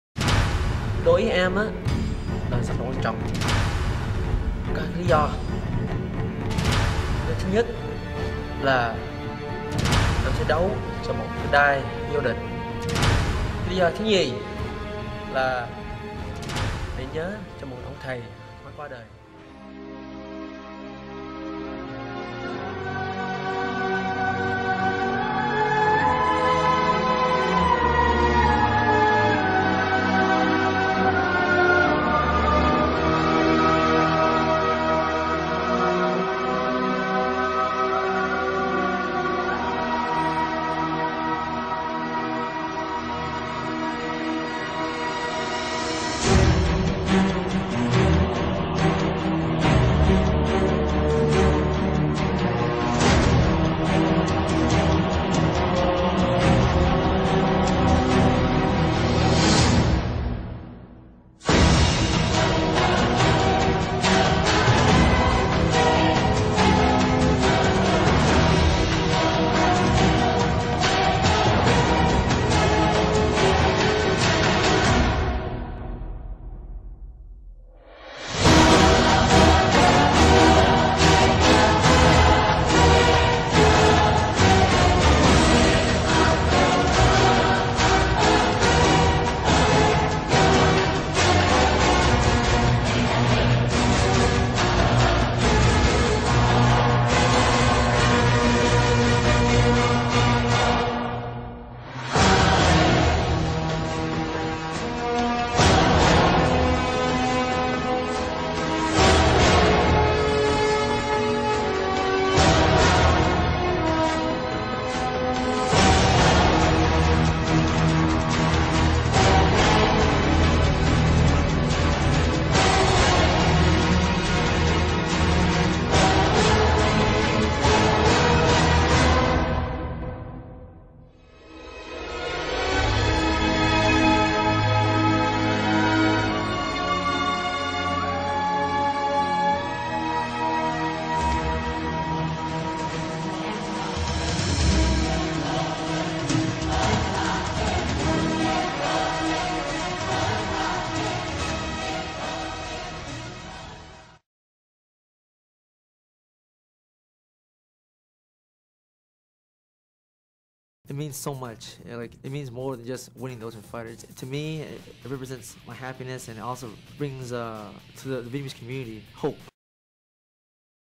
Nam Phan (Kevin Anderson Highlight)
Watch this in Full Screen mode, or the giant blackness surrounding the video will probably irritate you.  Thank You.

This is my first highlight ever.  It's for a fighter who is currently on season 12 of The Ultimate Fighter.  He's one of the most down to earth guys, check it out and follow his fights on TUF (10 PM on Spike TV).  Keep in mind this is my first highlight and I know the music cut off a little early, but I don't think it's a huge deal.